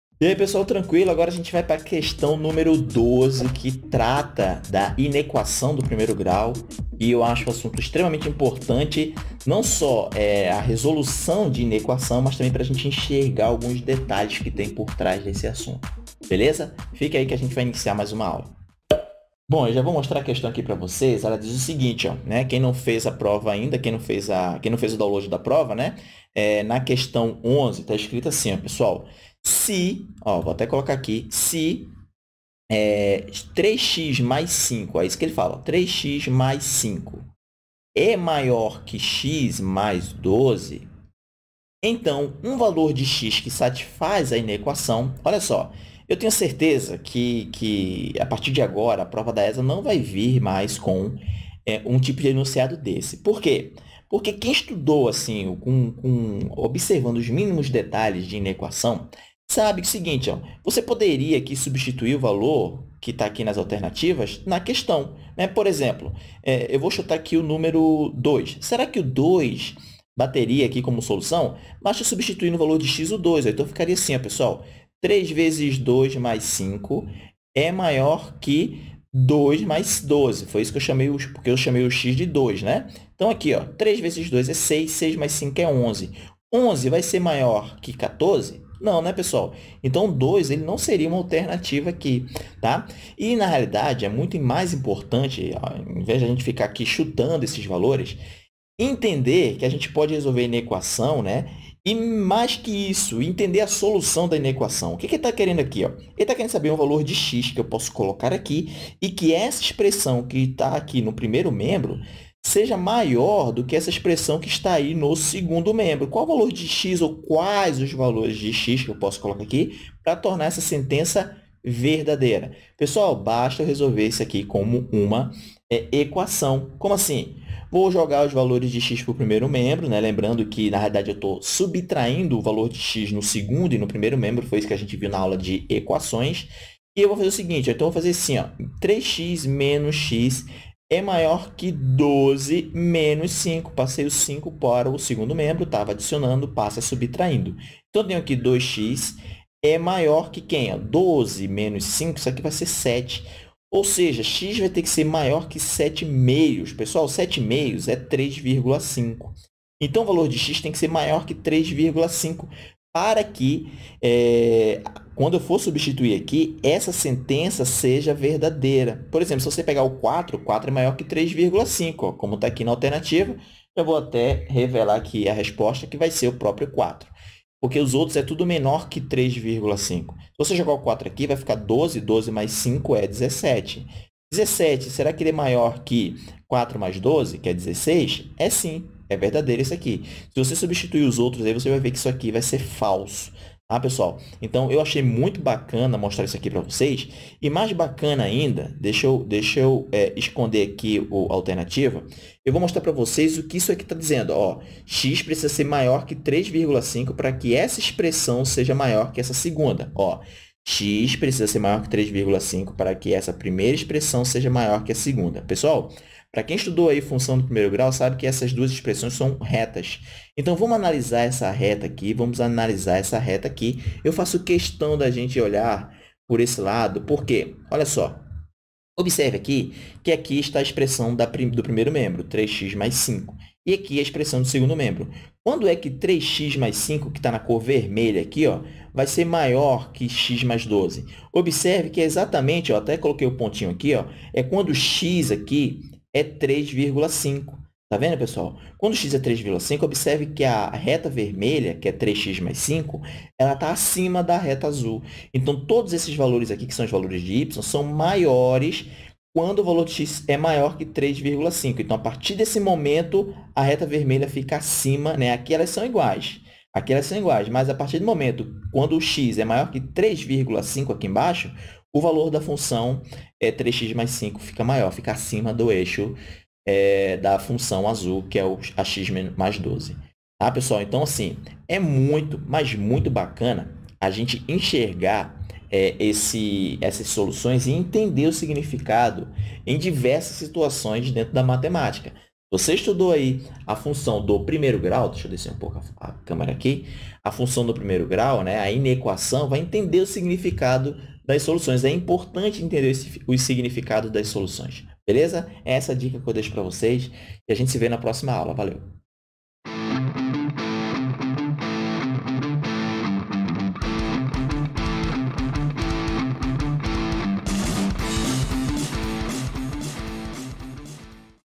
ESA1982 - Q11 - Inequação do 1º grau - Matemática Básica | Seja Militar
00:00 Inscreva-se e seja feliz
00:20 Solução da Q11

A Inequação do 1º grau é extremamente importante no entendimento do sinal da função e entender sua solução é algo que você, sargento, precisa saber. Construa sua base da melhor maneira possível estudando essas questões, para aprender propriedades novas e ganhar confiança. Não se desespere, pois se eu consegui você também vai, apenas resolva junto comigo essas questões de matemática e se torne um militar. Combinado?